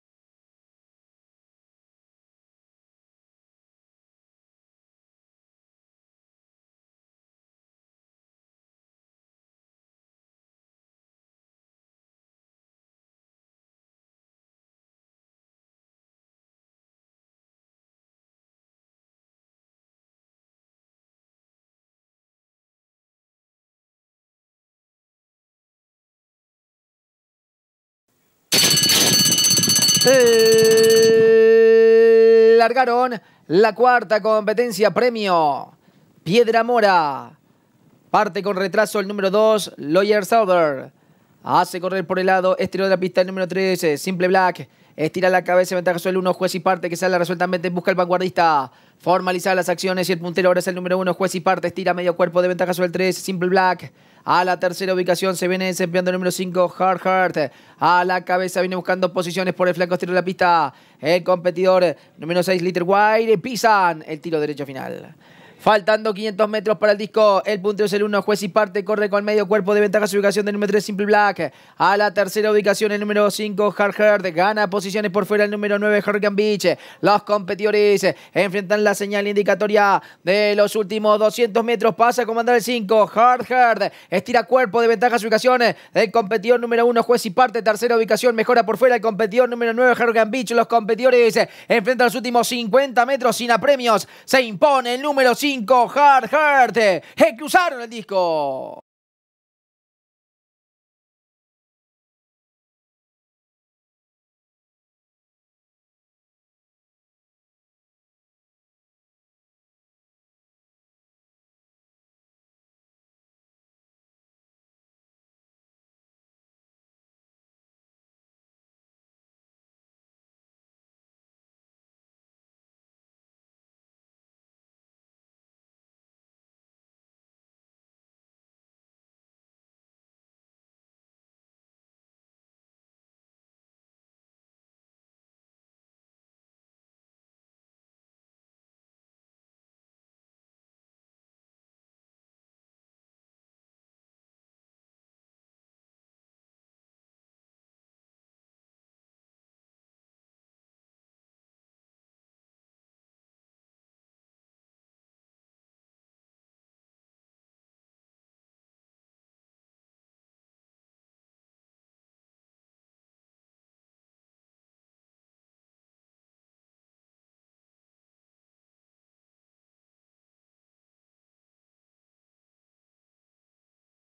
03/02/2017 – Hipodromo Maroñas – Carr 4 – PIEDRA MORA (2012)
Carrera 4 realizada el dia 03/02/2017 por el premio PIEDRA MORA (2012) en el Hipodromo Maroñas